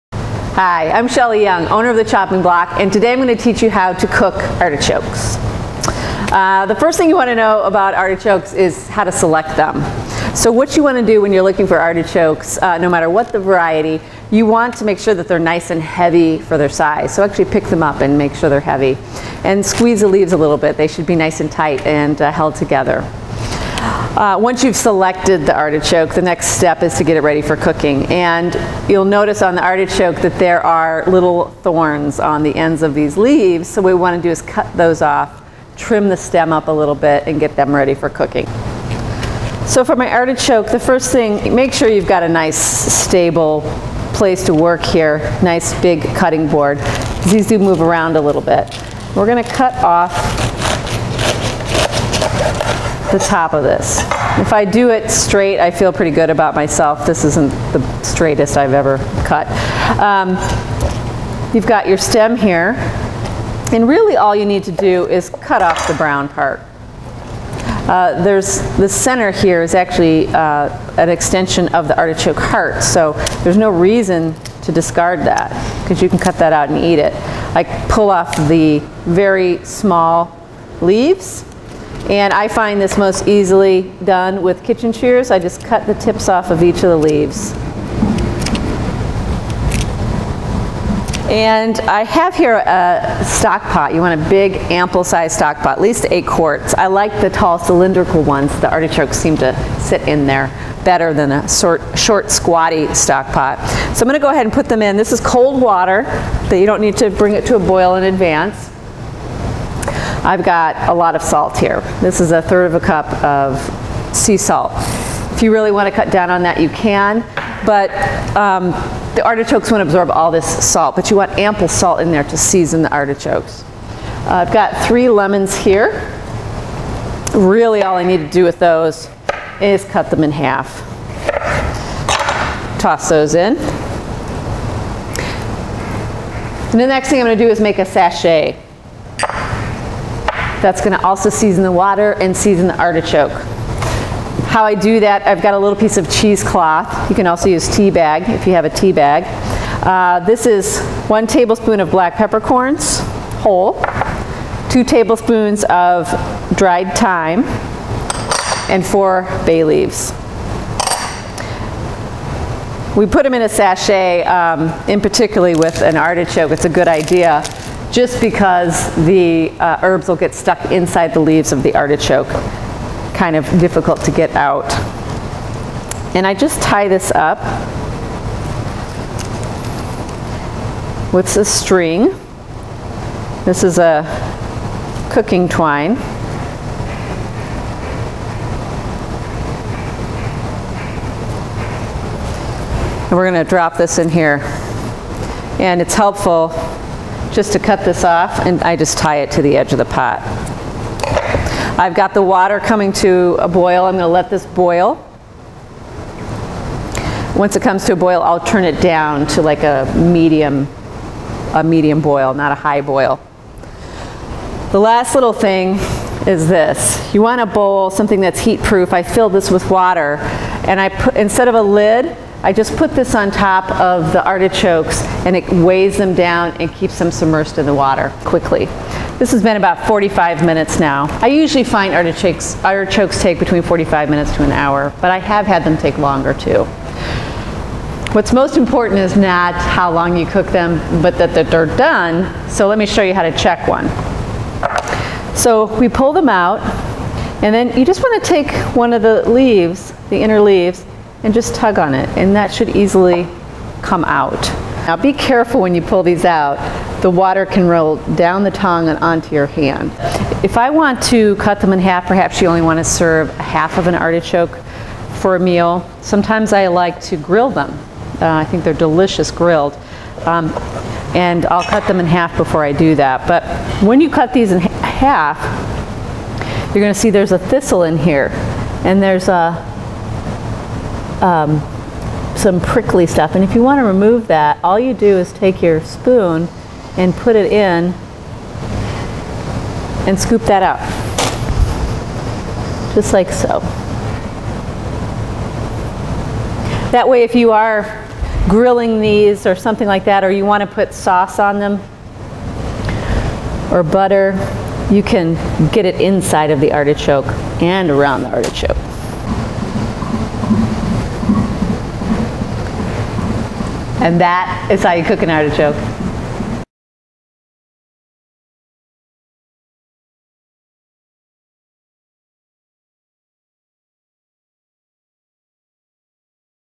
How to Cook Artichokes
The Chopping Block's Owner Chef Shelley Young demonstrates how to cook artichokes.

4 artichokes, stem trimmed, top of artichoke cut off, top of leaves trimmed with kitchen shears
3 lemons, halved
1/3 cup salt
Sachet: 1 Tablespoon whole black peppercorns, 2 Tablespoons dried thyme, 4 bay leaves (tie together with cooking twine in cheesecloth)

Place all ingredients in large stock pot of cold water. Place a heatproof bowl filled with water on top to weigh everything down. Heat to boil then reduce heat to medium. Cook for 45  - 60 minutes or until a leaf in the center can be removed and is soft. Slice artichokes in half. Remove thistle from center. Serve with melted butter.